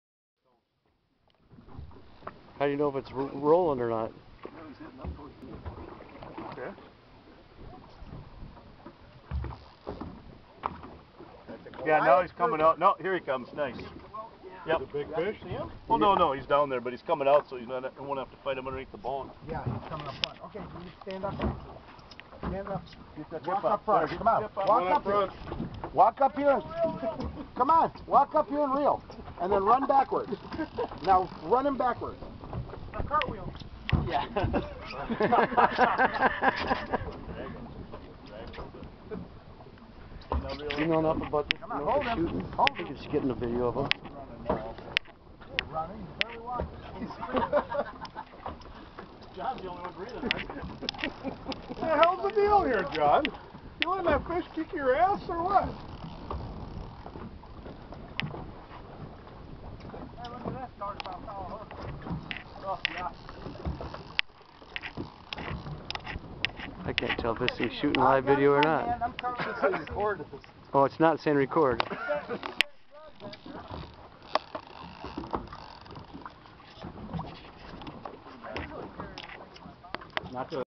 Jon Sandholm meets his match
A 400 lb Grouper can't stop Jon Sandholm.